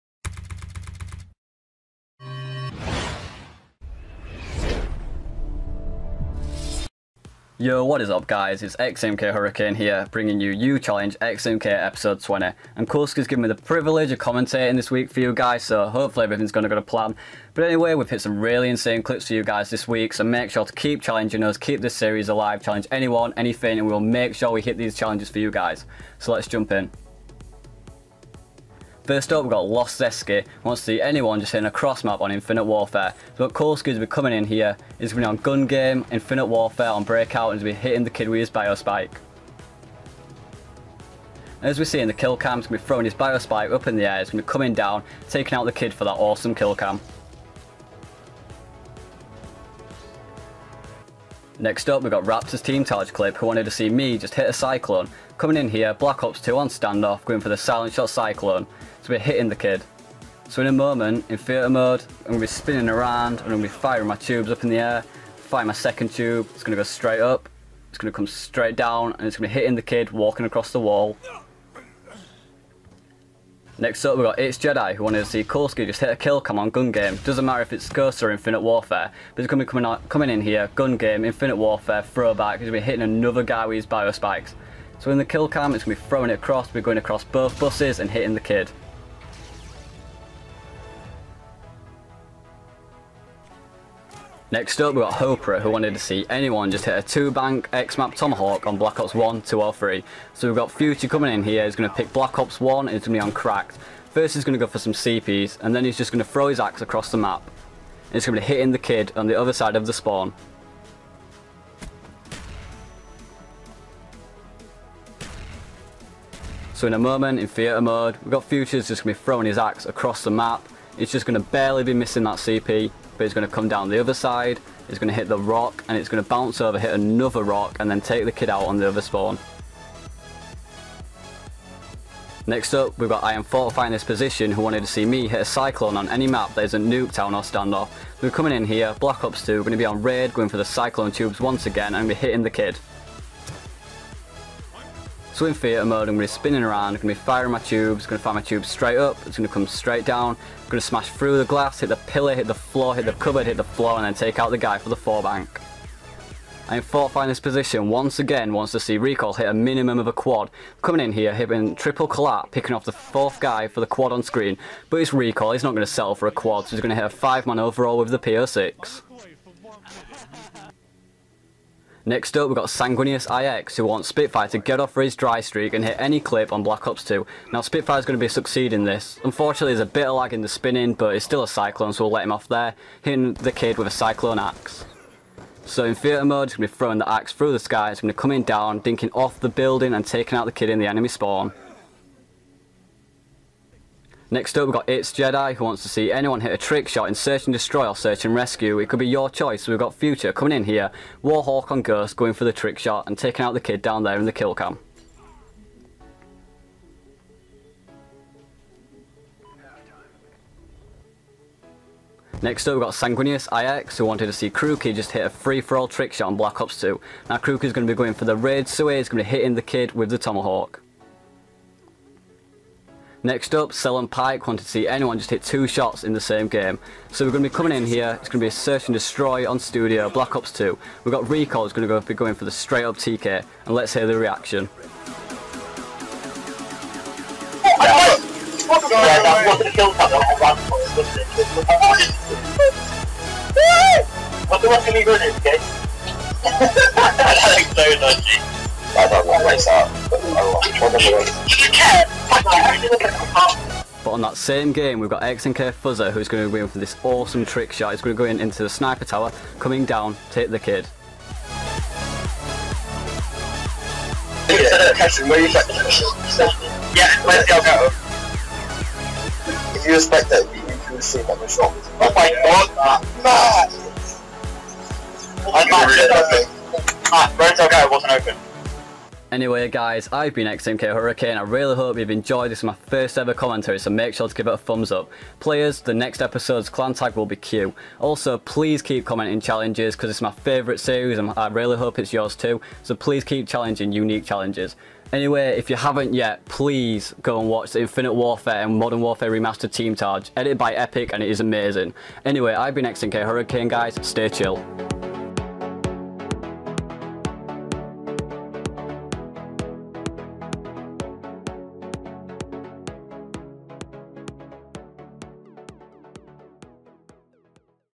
You Challenge xAMK #20 | [Q] w/ xAMK Hurricane
Hey guys, today we have another episode of You Challenge xAMK! Episode 20! Commentated by xAMK Hurricane and we have some amazing challenges for you guys! Hope you enjoy and please leave us some challenges to hit and we will try get another long episode out as members are now on their Christmas break so even more challenges can be hit!

Thumbnail By xAMK Coolski